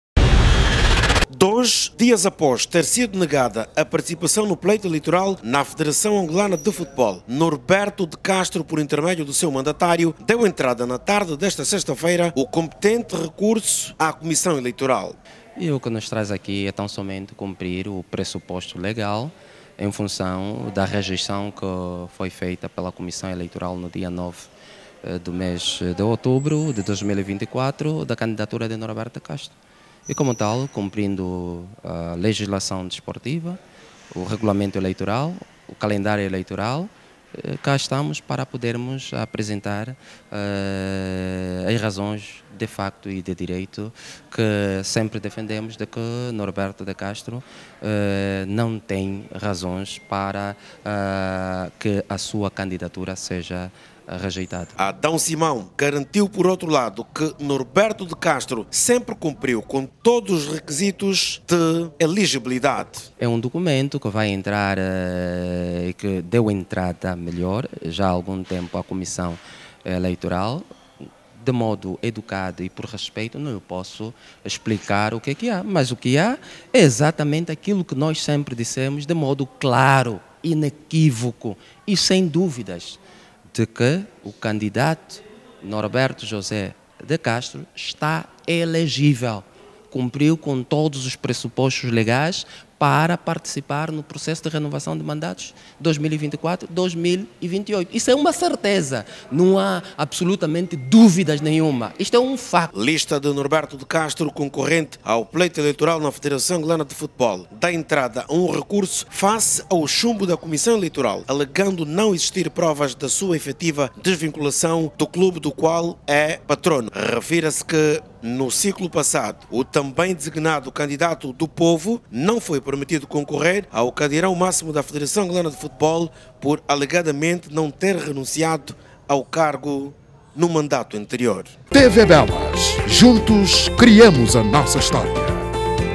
ELEIÇÕES NA FAF CANDIDATURA DE NORBERTO DE CASTRO APRESENTA RECURSO
Dois dias após ter sido negada a participação no pleito eleitoral na federação angolana de futebol, Norberto de Castro, por intermédio do seu mandatário, deu entrada na tarde desta sexta-feira, o competente recurso à comissão eleitoral,Adão Simão, garantiu por outro lado, que Norberto de Castro, sempre cumpriu com todos os requisitos de elegibilidade.

Lista de Norberto de Castro, concorrente ao pleito eleitoral na FAF, dá entrada a um recurso, face ao chumbo da comissão eleitoral, alegando não existir provas da sua efectiva desvinculação do clube do qual é patrono.
Refira-se que, no ciclo passado, o também designado candidato do povo, não foi permitido concorrer ao cadeirão máximo da FAF, por alegadamente não ter renunciado ao cargo no mandato anterior.Saiba mais no